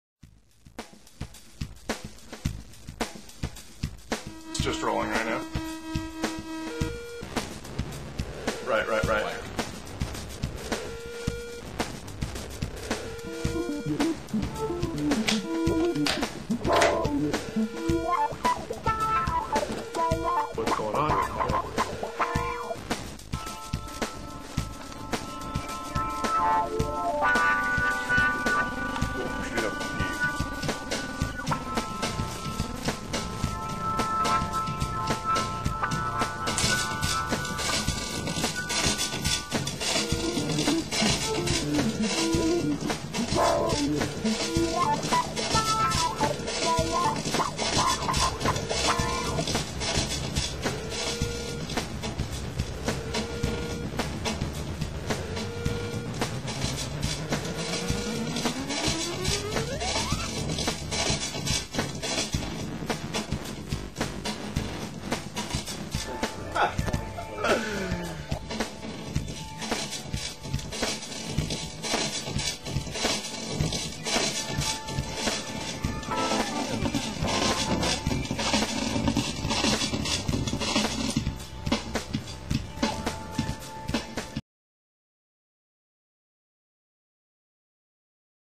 Tonebutcher Demo
Tonebutcher Custom Effect Pedals, Video mash up of pedals being used and tones getting abused.